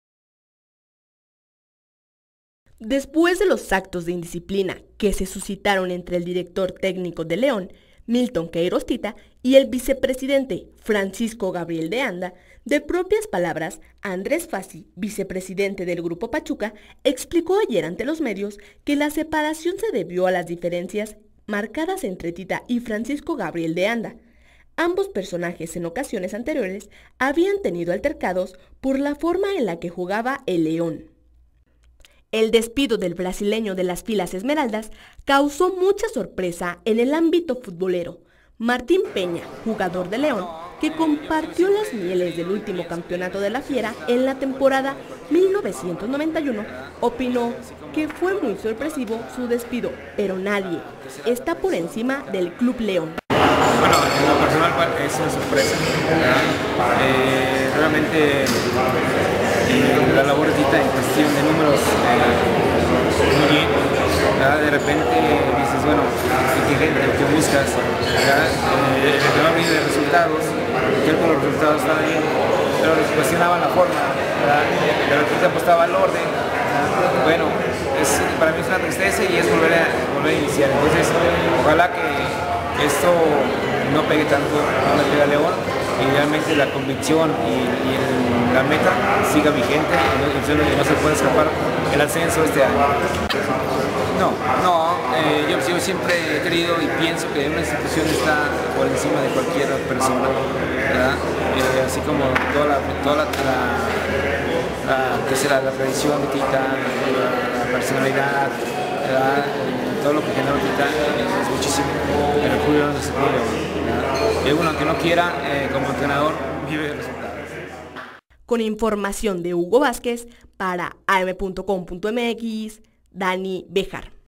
Si hubo pleito entre "tita" y Francisco Gabriel de Anda
Vea crónica en am.com.mx  Francisco Gabriel de Anda aseguro que si tenia fricciones con el brasileño